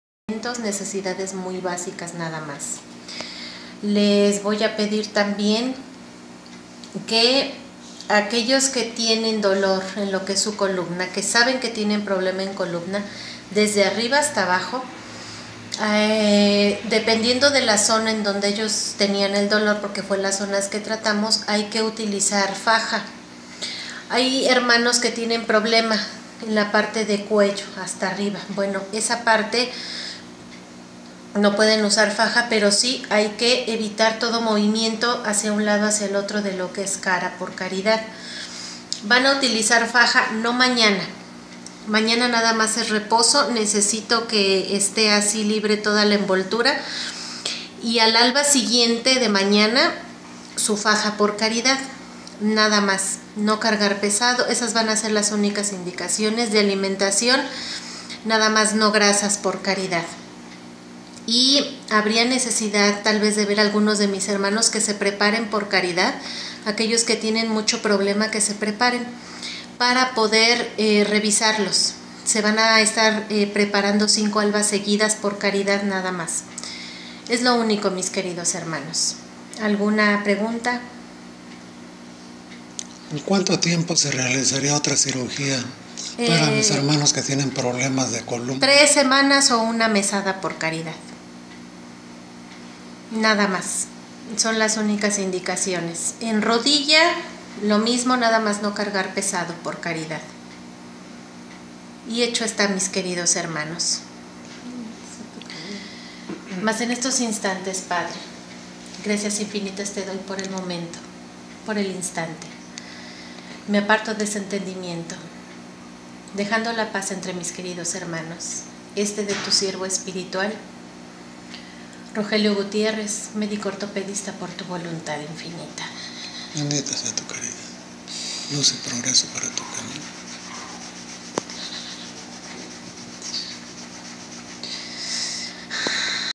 RECOMENDACIONES DESPUÉS DE CIRUGÍA
Recomendaciones después de la cirugía de columna y ortopedia, el viernes 15 de mayo de 2020.